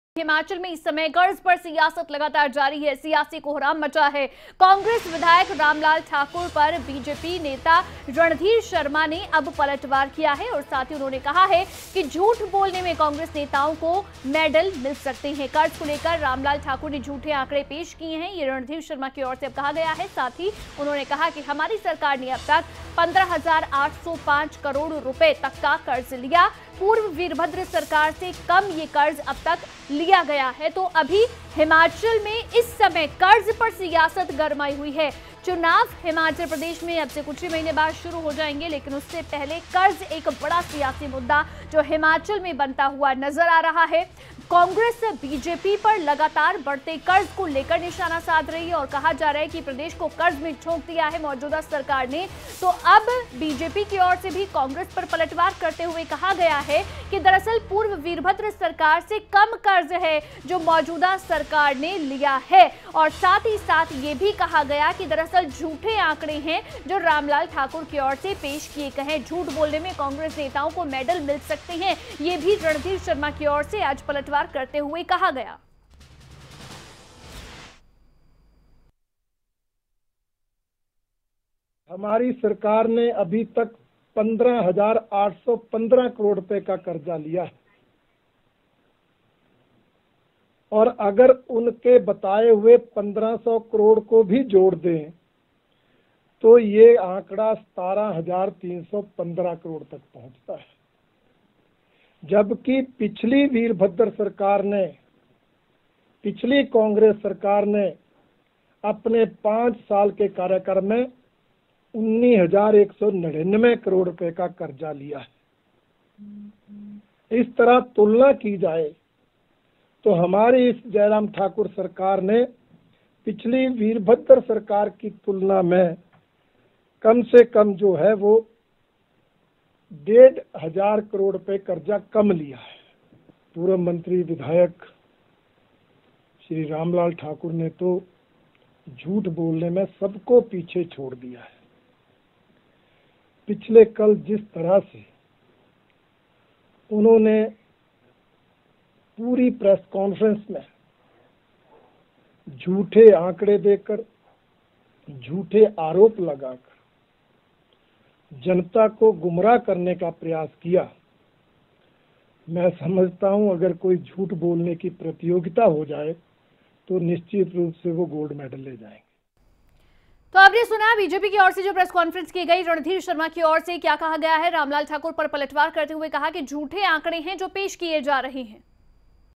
कर्ज पर सियायत जारी: कांग्रेस विधायक रामलाल ठाकुर पर बजेपी नेता रणधीर शर्मा का तीखा जुबानी वार !!
Himachal : कर्ज पर सियायत जारी, कांग्रेस विधायक रामलाल ठाकुर पर बजेपी नेता रणधीर शर्मा का वार